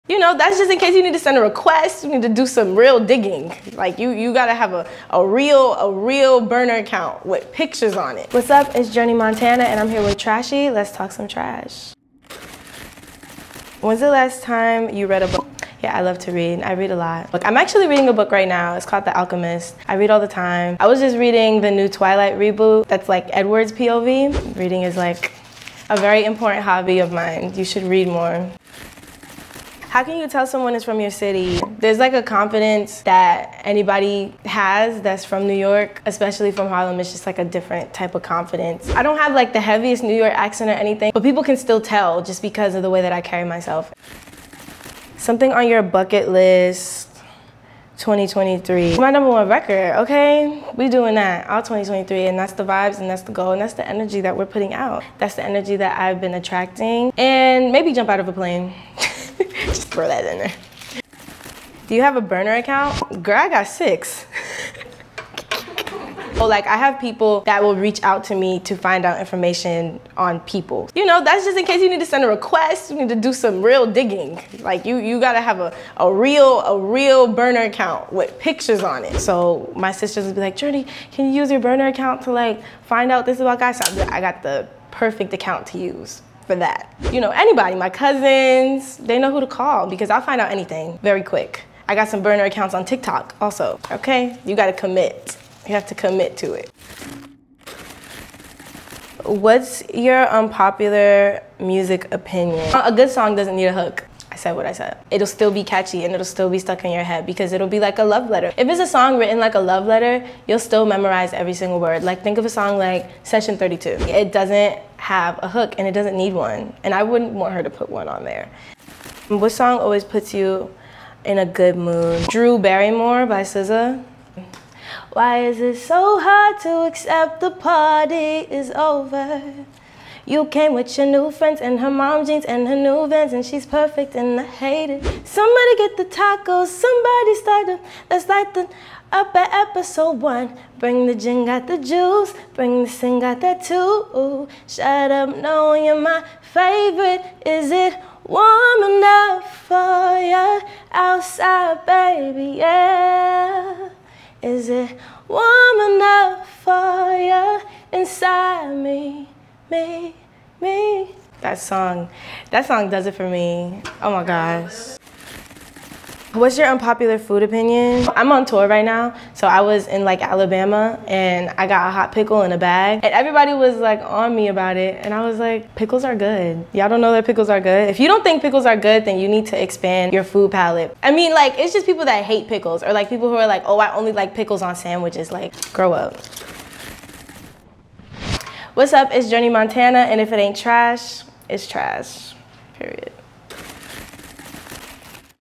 Journey Montana Talks Having 6 Burner Accounts With A Trash Can! | TRSH Talk interview
@JourneyMontana sits with TRSHY to talk about how she uses her burner accounts, the importance of reading, being from New York and so much more in this fun interview!

IF IT AINT TRSH, ITS TRASH!